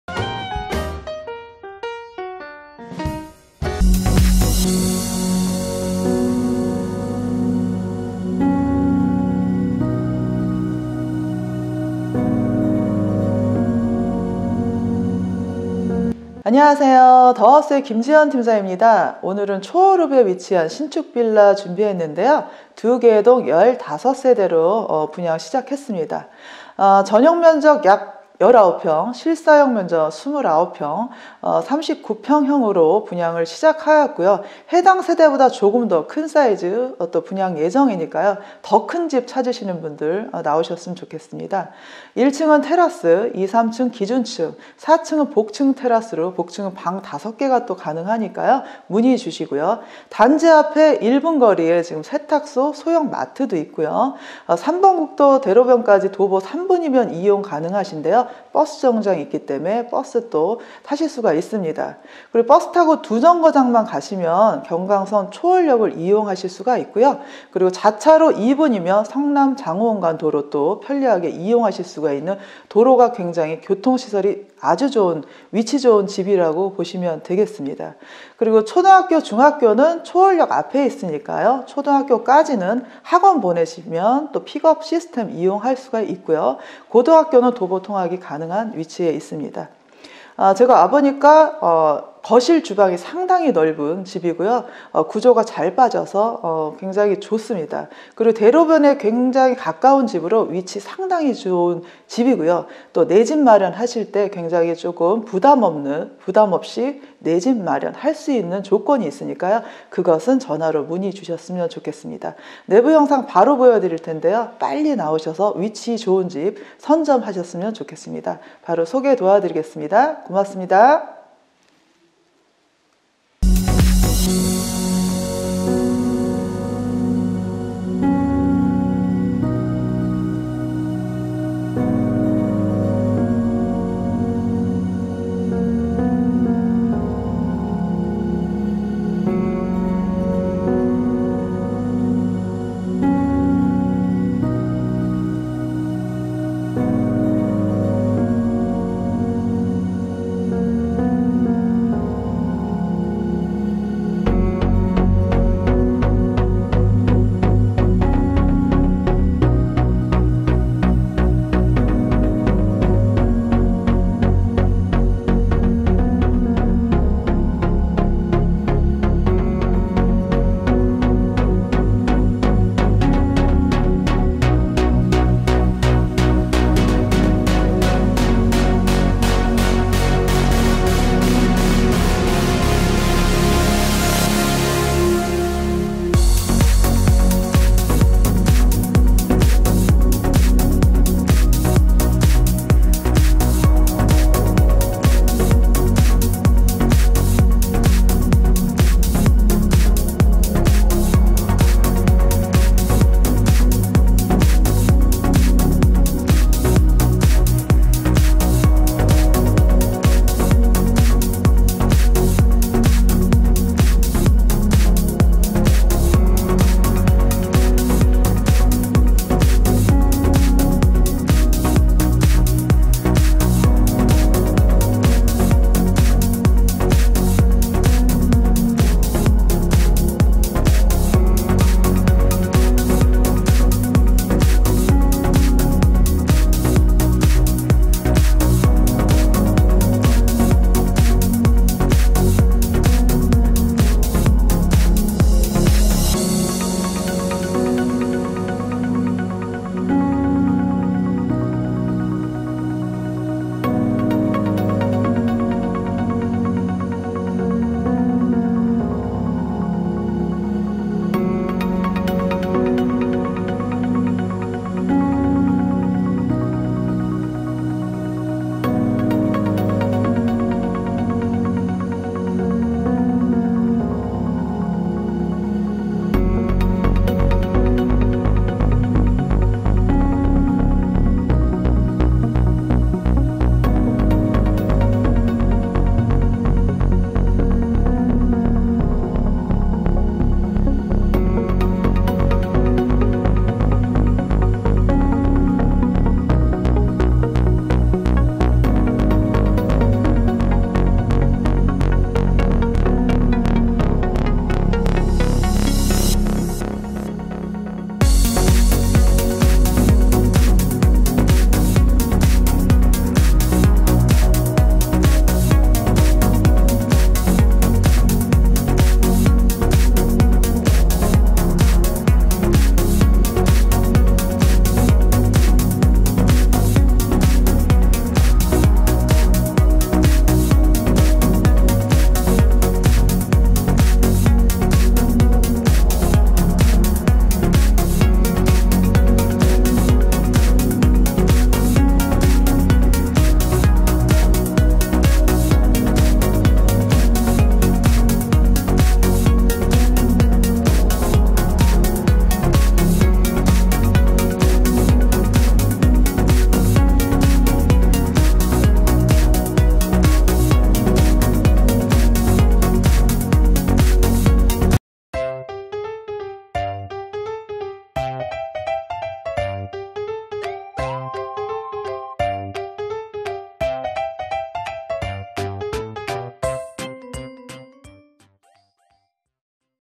[분양완료]경기광주신축빌라/ 초월읍신축빌라 위치 좋은 남향 집!!초월역 3km, 광주 원주간 도로 자차 2분[매물번호 321번]
▣ 경강선 초월역 3km, 자차 5분, 버스로 2정거장 위치
     삼동역, 경기광주역,이매역, 판교역, 강남역 : 생활권 가능
▣ 1층 테라스, 2, 3층 기준층, 4층 복층 테라스 구조
▣ 전용면적 약19평, 실면적 약29평, 39평형 분양 중입니다

▣ 2개동 15세대 신규 신축빌라 오픈!!

▣ 단지 앞 세탁소, 소형 마트 30초~~

▣ 방3개, 욕실2개, 큰 거실, 큰 주방, 다용도실

▣ 초월역 앞~ 도곡초, 초월중 ▣ 고등학교 도보 통학 가능

▣ 상담 문의 & 방문 예약 : [매물번호 321번]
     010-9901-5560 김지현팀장

＊다른 현장과의 시세 비교는 무료 투어로 만나요~

＊첫 만남이 매우 중요합니다~

＊내 가족이 산다는 마음으로 도움을 드리겠습니다 ^^